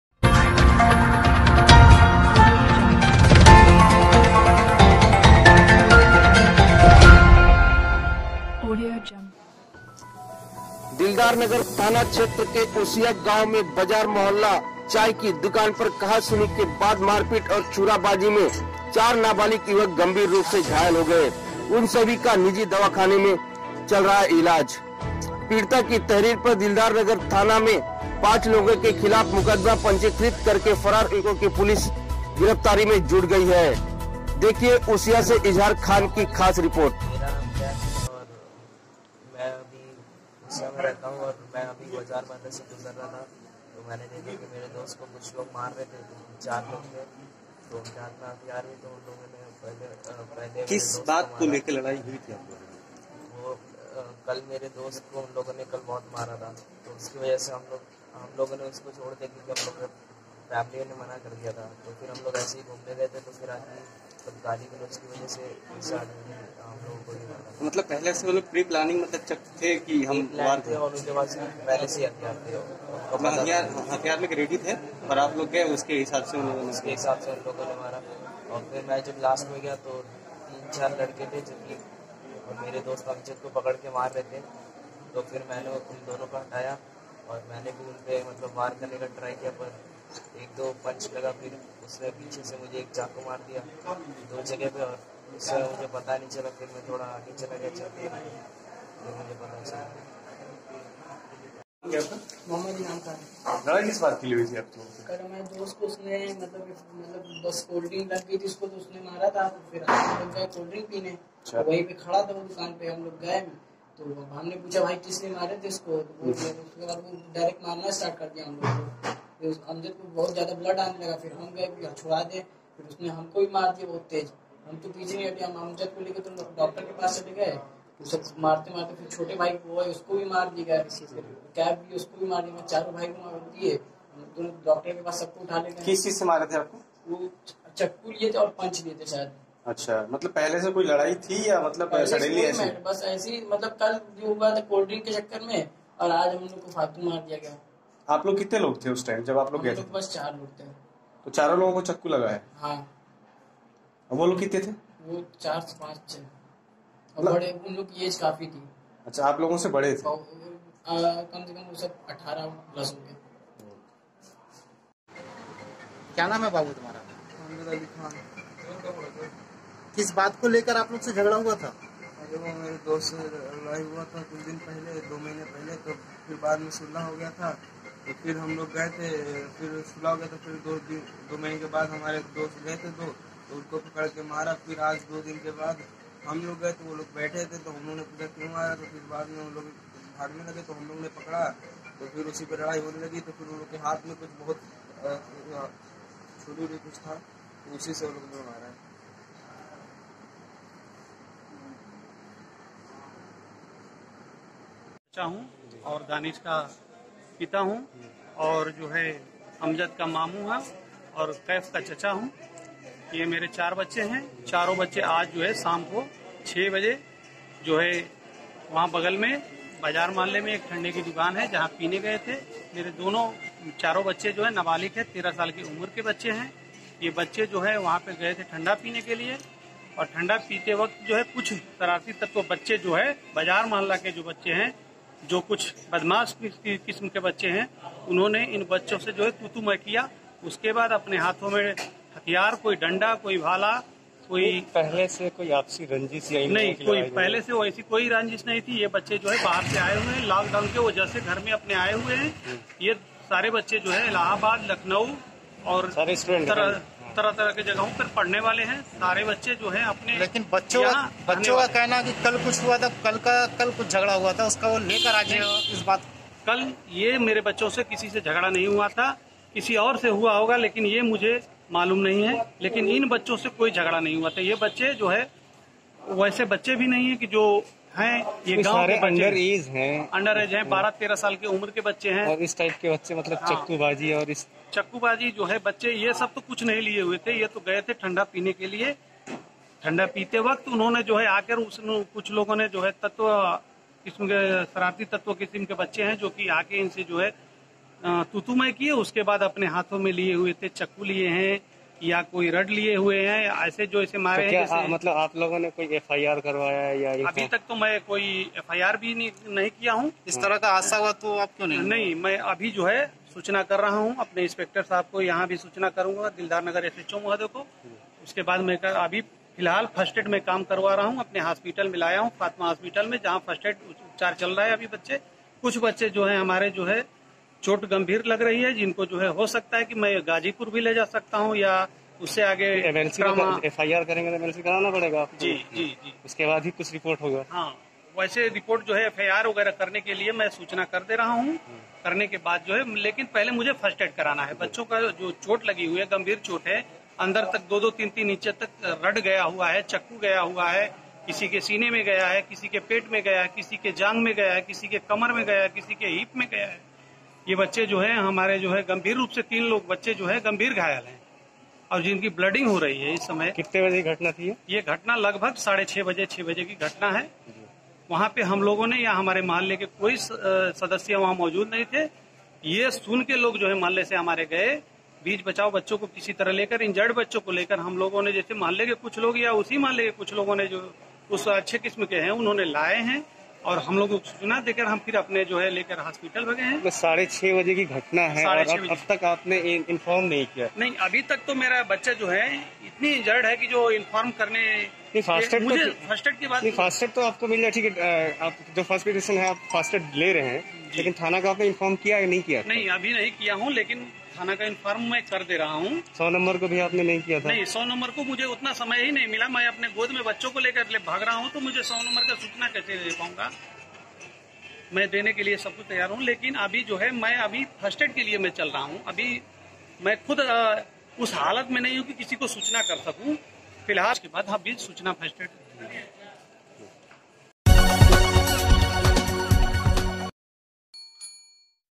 BreakingNews:खुलेआम बाजार में चला छुरा चार नाबालिक घायल
दिलदारनगर थानां के उसिया गांव के बाजार मोहल्ला में चाय की दुकान पर कहासुनी के बाद मारपीट और छुरा बाजी में चार नाबालिक युवक गंभीर रूप से हुये घायल  उन सभी का निजी दवाखाने में चल रहा है ईलाज
पीड़ित की तहरीर पर दिलदारनगर पुलिस थाने में 5 लोगो के खिलाफ मुकदमा पंजीकृत करके फरार युवकों के गिरफ्तारी में पुलिस जुट गई है।